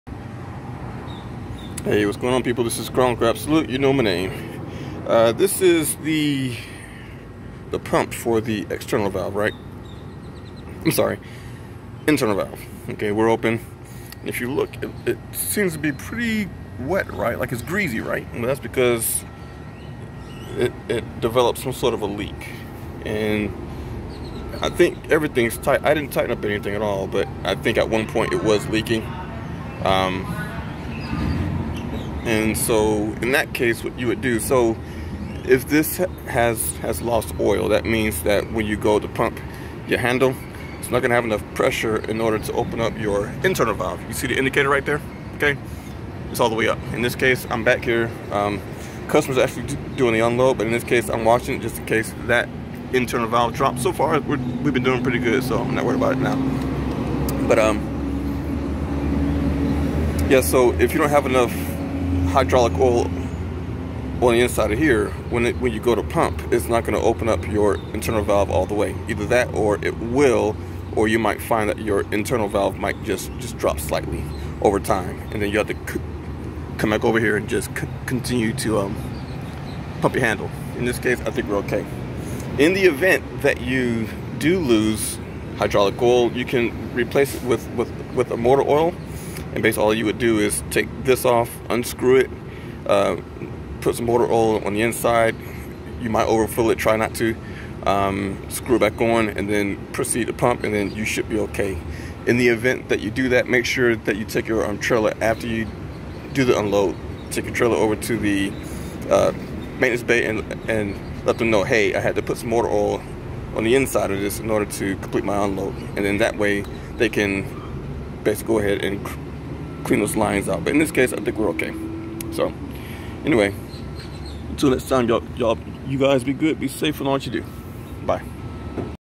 Low pressure on internal valve?
A pretrip on an empty trailer before its loaded will give you time to find a good fix on this. However if you are picking up a relay on a loaded trailer, you just wont know if the internal is working properly. If you dont have enough oil in your internal valve lines, your internal valve will slowly drop. Sometimes it wont open at all. A quick fix is to pour in some engine oil, complete the unload, then take the trailer to the shop so they can clear the lines. Hope this helps.